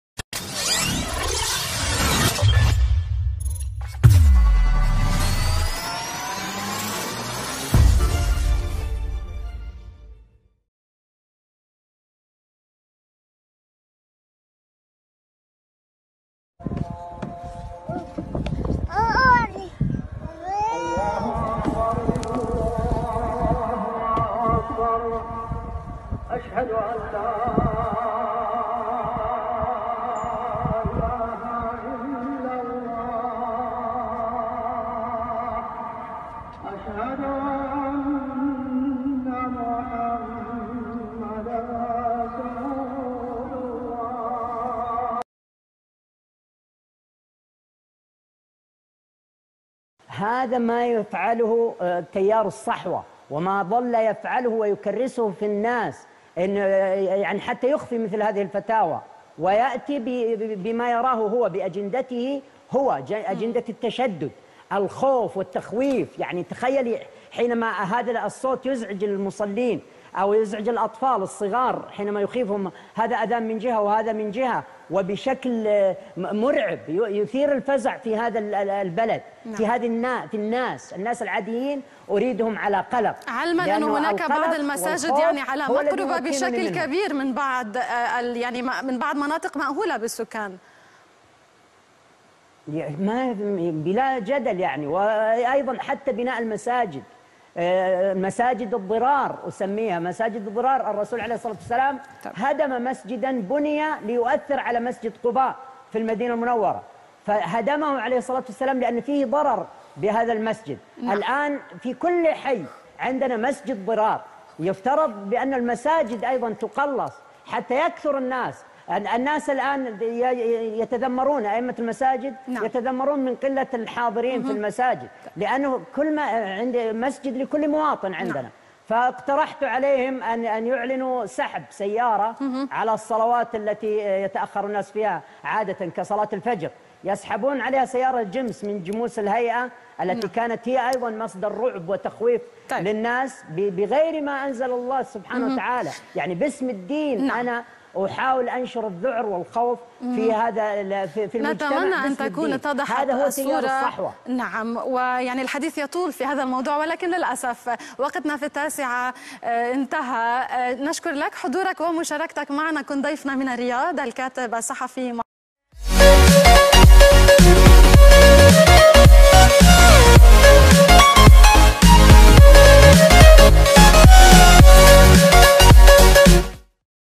شوف كيف الاذان يرعب الاطفال ويخوفهم
شاهد طريقة رعب الاطفال وخوفهم عند سماع الاذان

لمشاهدة باقي الفيدوهات